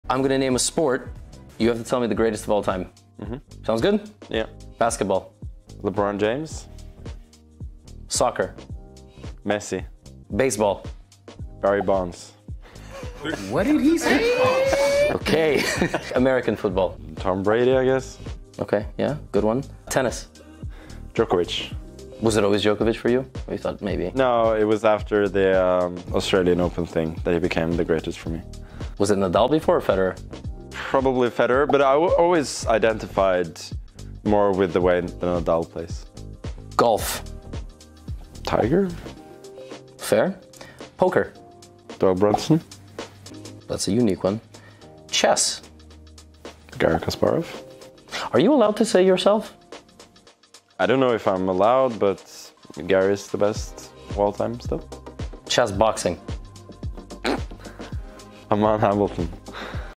Magnus Favorite Sports GOAT 😎🐐
In this video, Magnus revealed his favorite sports GOAT of all the other sports. Enjoy.